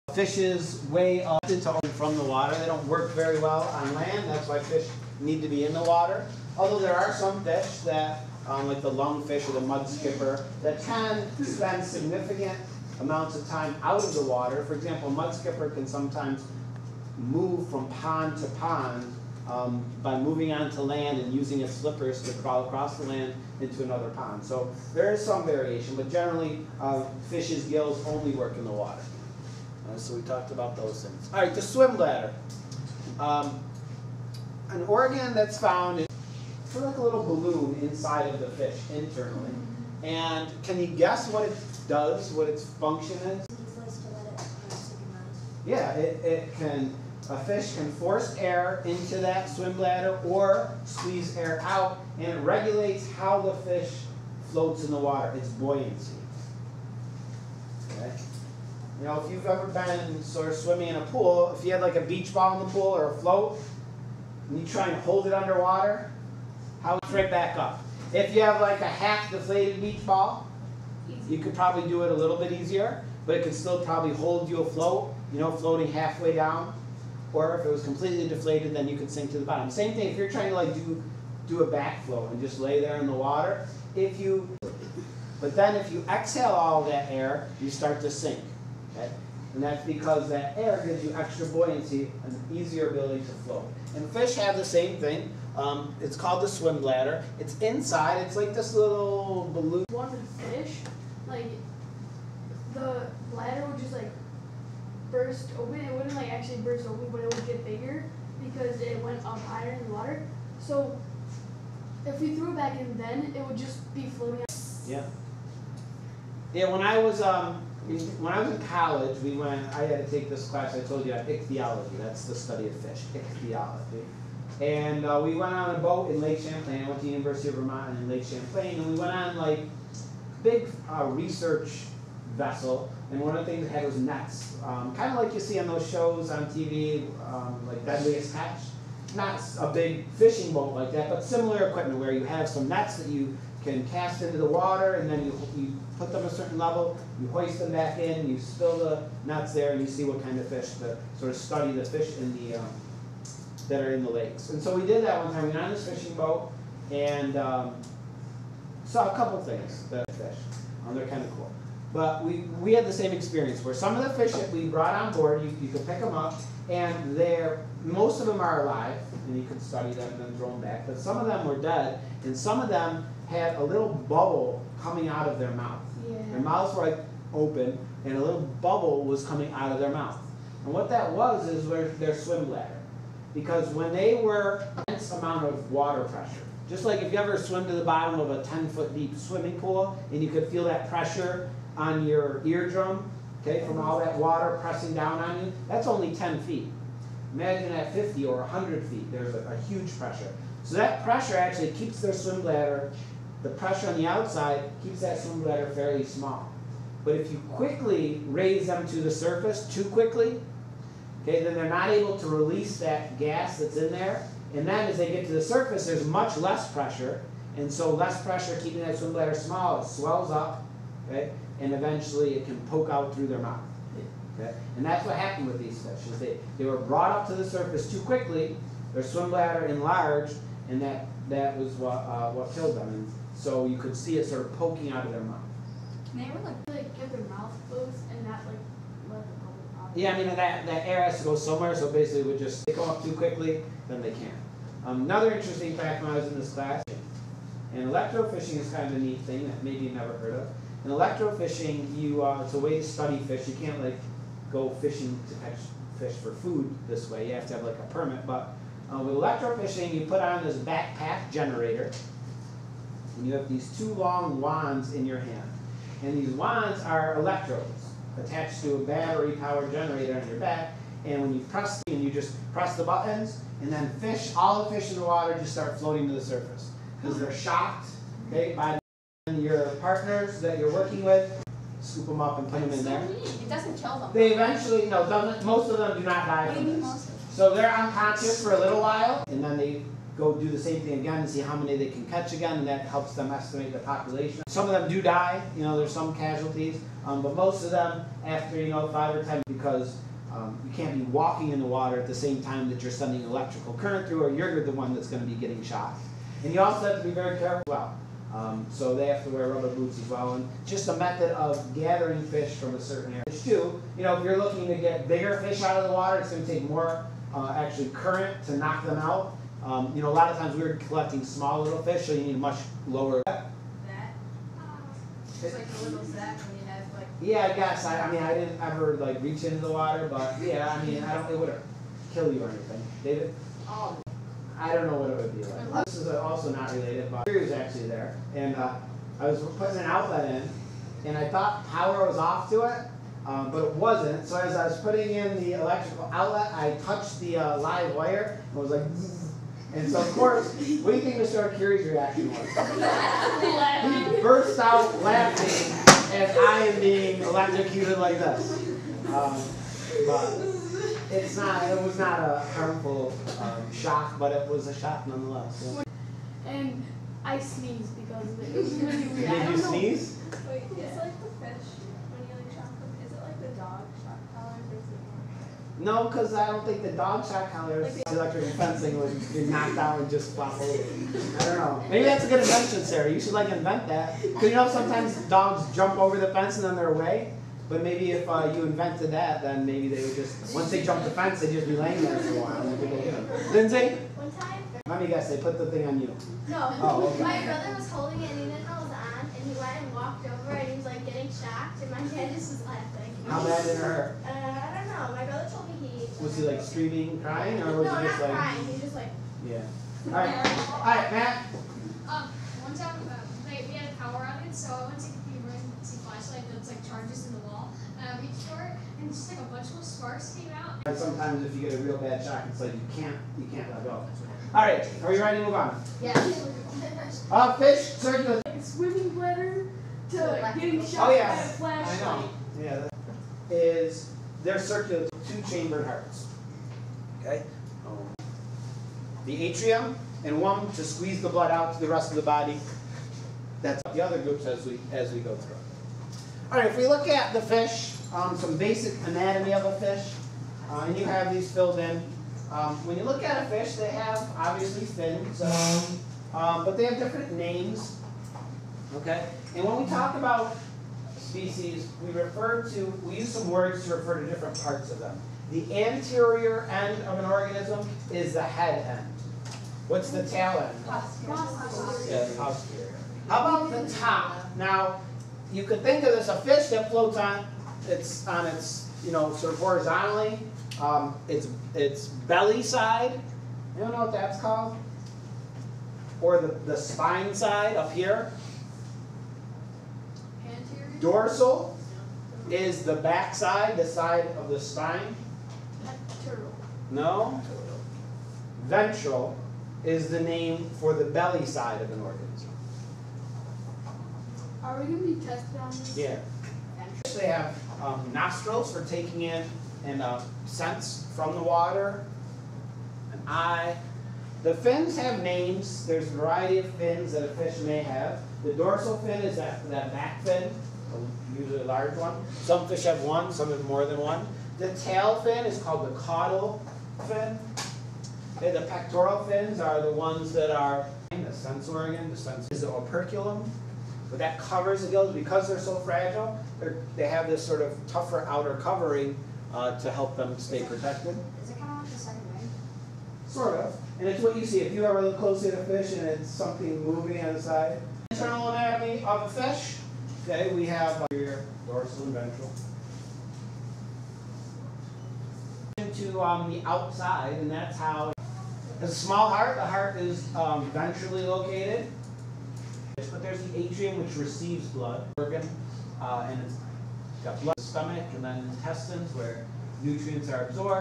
Fish - Part 2
In this video we cover some fish adaptations and also some basic fish anatomy